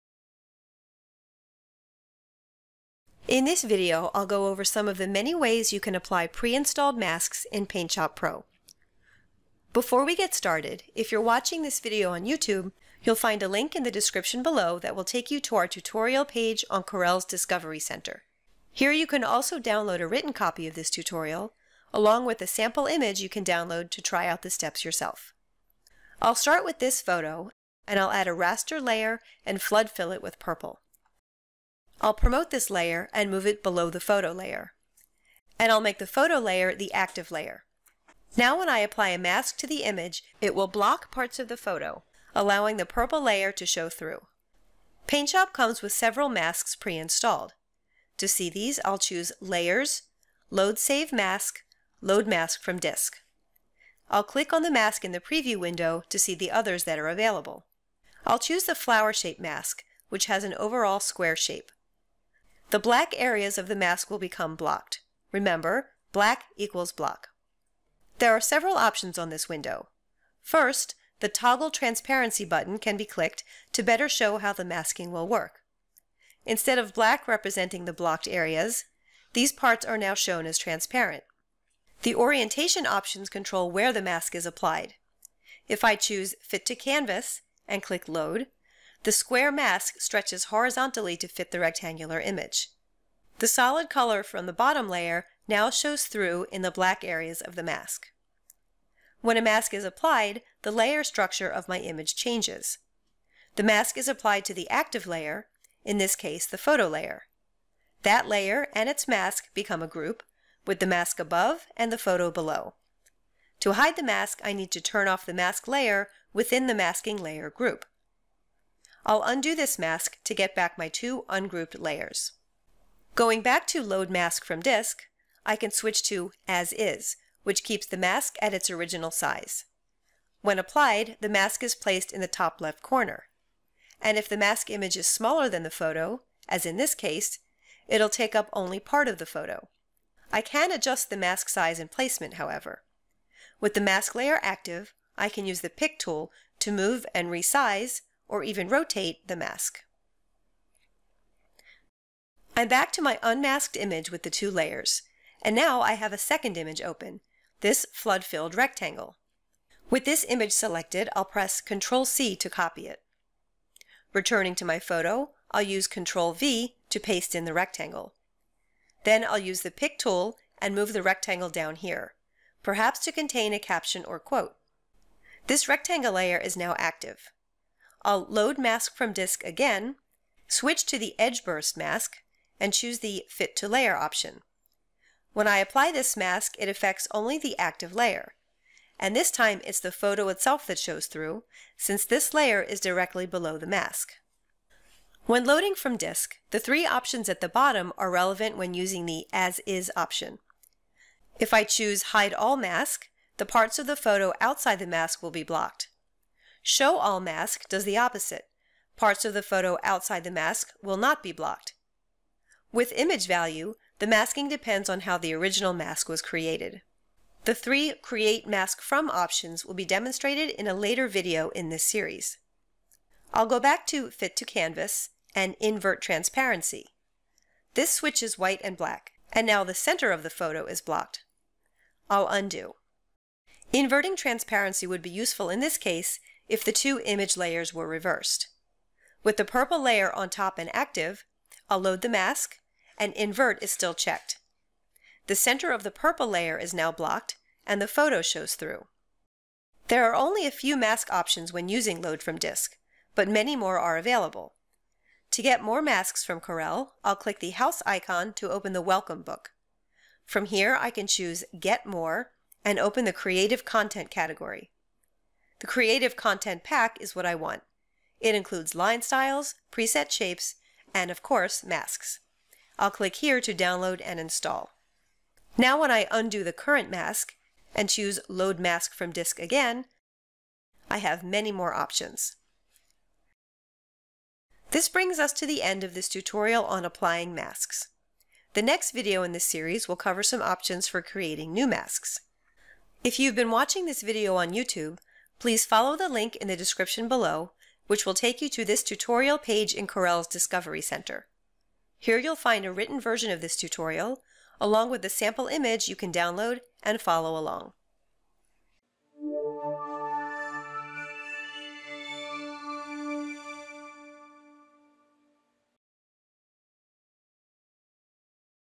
How to Apply Masks in PaintShop Pro
Learn how to apply and adjust a simple mask from PaintShop Pro’s mask library, and how to get free masks and more creative content from the Welcome Book. Download a printable copy of the tutorial plus sample image from our tutorial